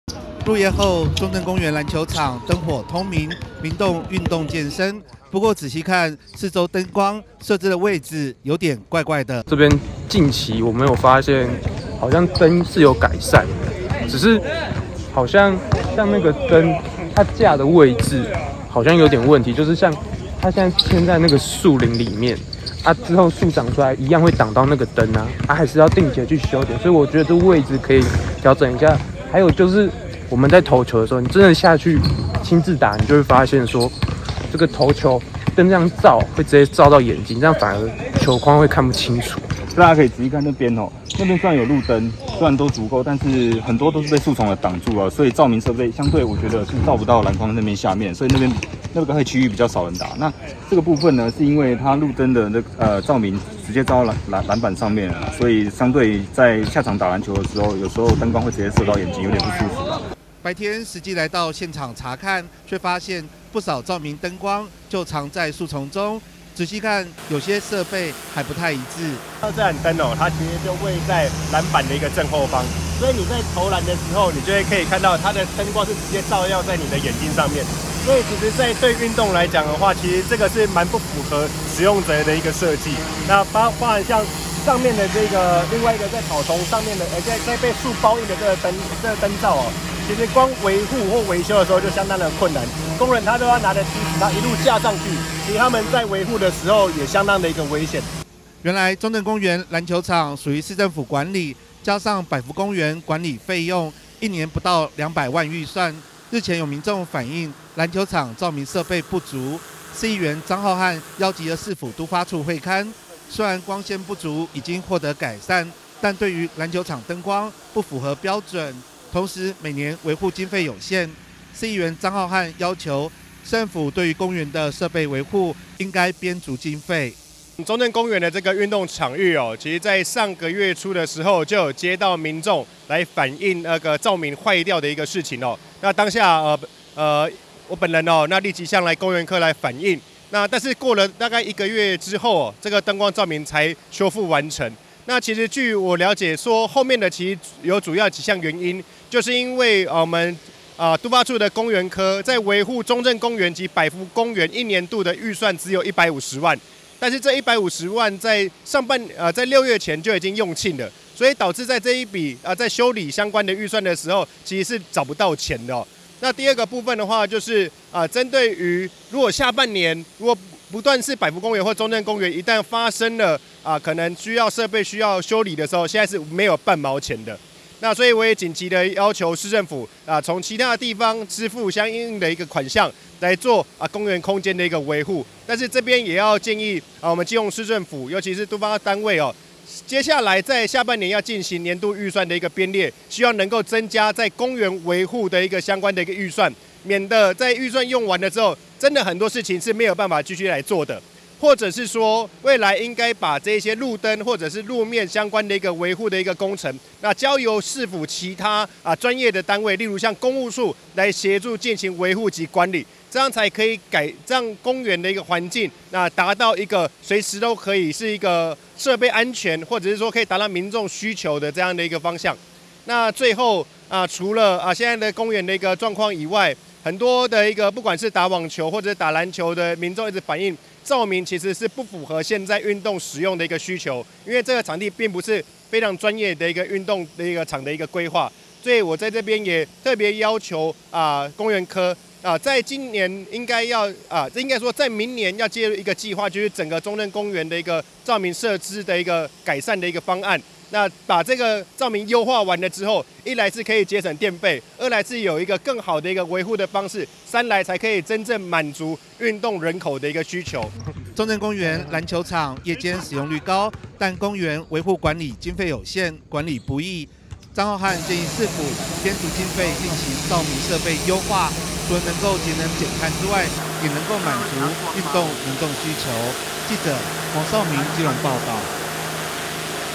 籃球場｜燈光｜好刺眼! 籃球場"燈光"不標準 影響視線投籃
(中嘉吉隆新聞 記者黃紹銘/基隆報導) 基隆中正公園籃球場，疫情解封後，不管是白天或夜晚，都有不少民眾在這運動，不過，有球友反映，籃球場設置的燈光位置，往往會直射眼睛，發生眩光情形，造成球友打球障礙，基隆市議員張顥瀚邀集市府單位會勘，希望市府能進行調整，並編列足夠經費維護公園設備。

入夜後，中正公園籃球場燈火通明，民眾運動健身，不過，仔細看，四周的燈光，架設的位置有點怪怪地。
白天實際來到現場勘查，卻發現，不少照明燈光，就藏在樹叢中，仔細看，有些設備的規格還不太一致。
原來中正公園籃球場，屬於市政府管理，但加上百福公園管理費用，一年不到200萬，日前民眾反映，籃球場照明設備不足，市議員張顥瀚邀集市府都發處會勘，雖照明設備不足已經獲得改善，但對於籃球場燈光設計，不符合標準，同時，每年維護經費有限，市議員張顥瀚要求，市政府對於公園設備維護，應該編足經費。
中正公園籃球場夜間使用率高，但公園維護管理經費有限，管理不易，議員張顥瀚建議市府，編列經費進行照明設備優化，除了能節能減碳外，也能滿足運動民眾需求。